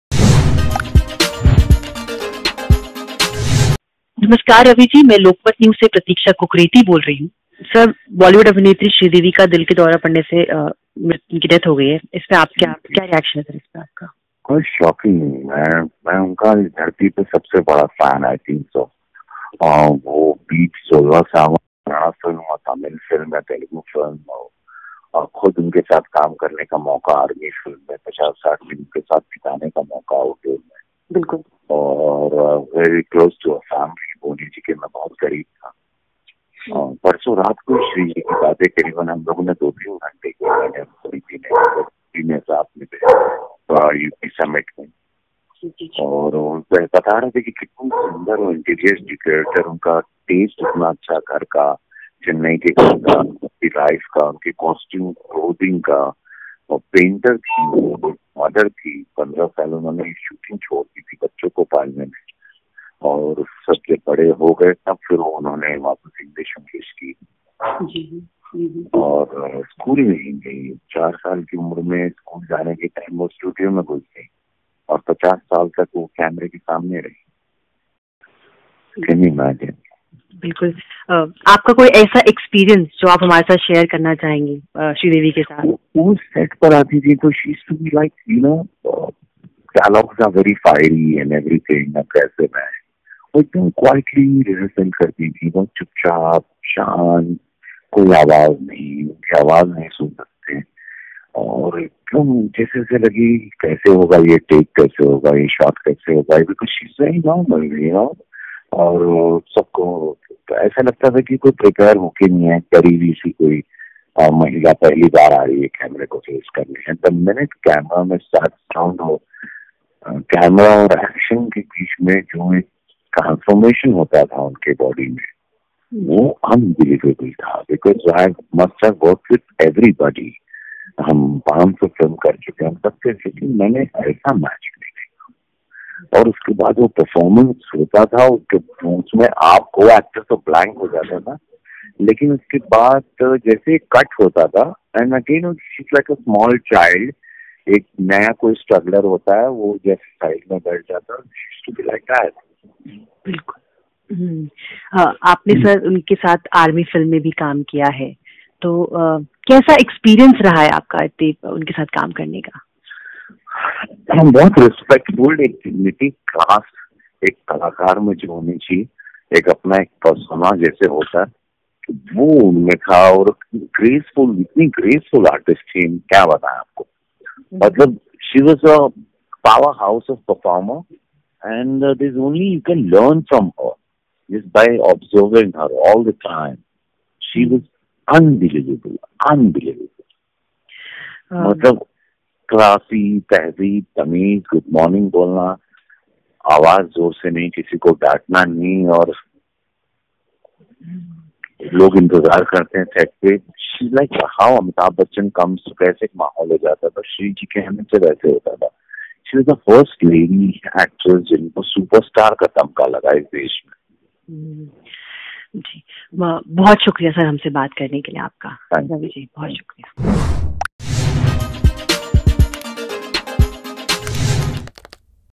Sridevi की मौत से सदमे में Ravi Kishan, Lokmat से की ख़ास बातचीत| Sridevi Died
बॉलीवुड की जानी-मानी अभिनेत्री श्रीदेवी का शनिवार (24 फ़रवरी) को हृदय गति रुक जाने से निधन हो गया। महज 54 साल की उम्र में श्रीदेवी ने दुनिया को अलविदा कहा है। उनकी मौत की खबर सुनकर बॉलीवुड सहित पूरा देश सदमे में है। श्रीदेवी का निधन दुबई में हुआ है लेकिन उनका अंतिम संस्कार मुंबई में ही किया जाएगा। इसके लिए उनका पार्थिव शरीर दुबई से मुंबई लाया जा रहा है।